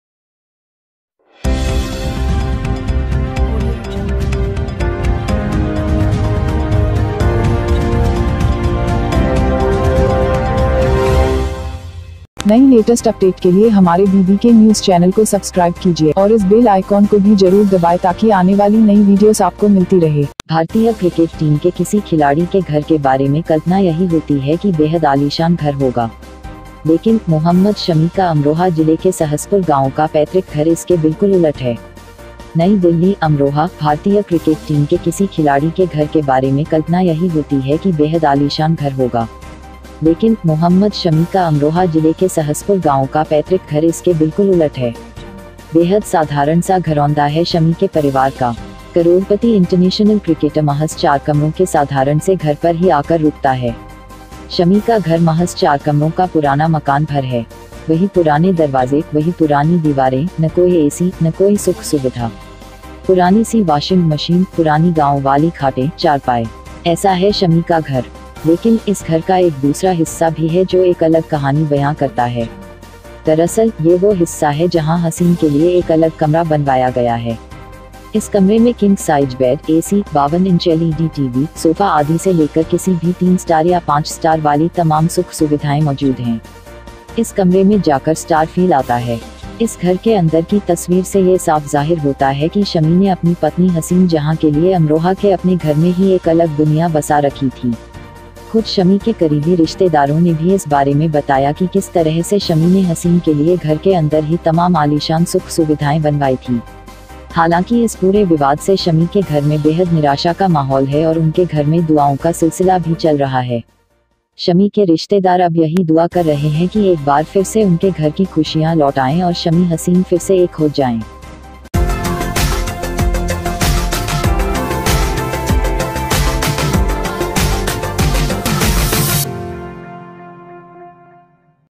सहसपुर के अपने घर में शमी ने बसा रखा है हसीन के लिए 'आलीशान जहां'
भारतीय क्रिकेट टीम के किसी खिलाड़ी के घर के बारे में कल्पना यही होती है कि बेहद आलीशान घर होगा. लेकिन, मोहम्मद शमी का अमरोहा जिले के सहसपुर गांव का पैतृक घर इसके बिल्कुल उलट है.

नई दिल्ली/अमरोहा: भारतीय क्रिकेट टीम के किसी खिलाड़ी के घर के बारे में कल्पना यही होती है कि बेहद आलीशान घर होगा. लेकिन, मोहम्मद शमी का अमरोहा जिले के सहसपुर गांव का पैतृक घर इसके बिल्कुल उलट है. बेहद साधारण सा घरौंदा है शमी के परिवार का. करोड़पति इंटरनेशनल क्रिकेटर महज़ 4 कमरों के साधारण से घर पर ही आकर रुकता है.
शमी का घर महज़ 4 कमरों का पुराना मकान भर है. वही पुराने दरवाज़े, वही पुरानी दीवारें, न कोई एसी, न कोई सुख सुविधा. पुरानी सी वाशिंग मशीन, पुरानी गांव वाली खाटें (चारपाई). ऐसा है शमी का घर.
लेकिन, इस घर का एक दूसरा हिस्सा भी है जो एक अलग कहानी बयां करता है. दरअसल, ये वो हिस्सा है जहां हसीन के लिए एक अलग कमरा बनवाया गया है. इस कमरे में किंग साइज बेड, एसी, 52 इंच एलईडी टीवी, सोफा आदि से लेकर किसी भी 3 स्टार या 5 स्टार वाली तमाम सुख सुविधाएं मौजूद हैं. इस कमरे में जाकर स्टार फील आता है.
इस घर के अंदर की तस्वीर से ये साफ ज़ाहिर होता है कि शमी ने अपनी पत्नी हसीन जहां के लिए अमरोहा के अपने घर में ही एक अलग दुनिया बसा रखी थी.
खुद शमी के करीबी रिश्तेदारों ने भी इस बारे में बताया कि किस तरह से शमी ने हसीन के लिए घर के अंदर ही तमाम आलीशान सुख-सुविधाएं बनवाई थी.